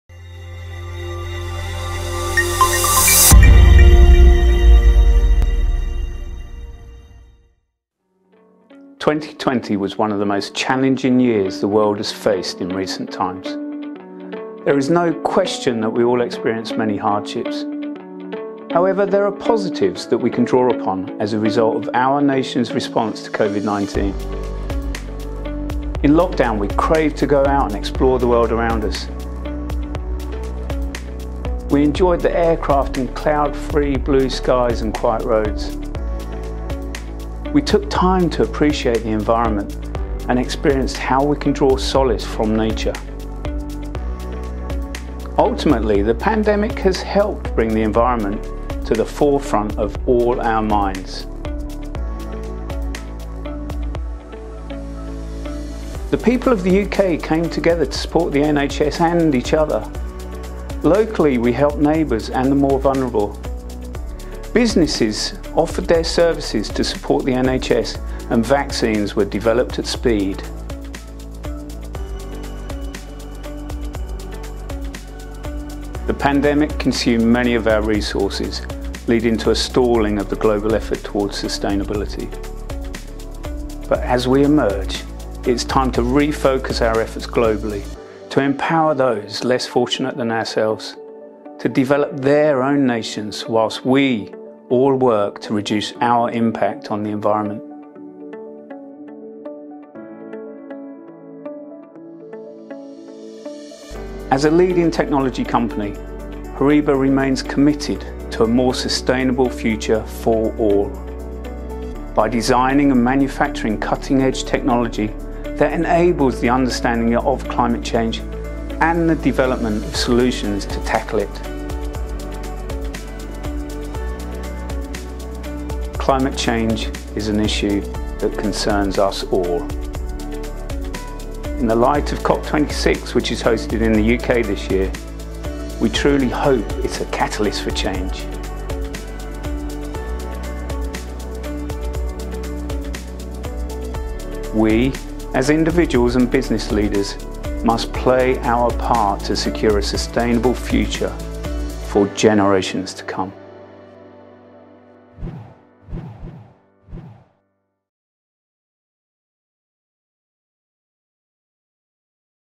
HORIBA - Our contribution to a sustainable future in light of COP26 - HORIBA UK
Climate change is an issue that concerns us all. In the light of COP26 which is hosted in the UK this year, we truly hope it is a catalyst for change.

As a leading technology company, HORIBA remains committed to a more sustainable future for all, by designing and manufacturing cutting-edge technology that enables the understanding of climate change and the development of solutions to tackle it.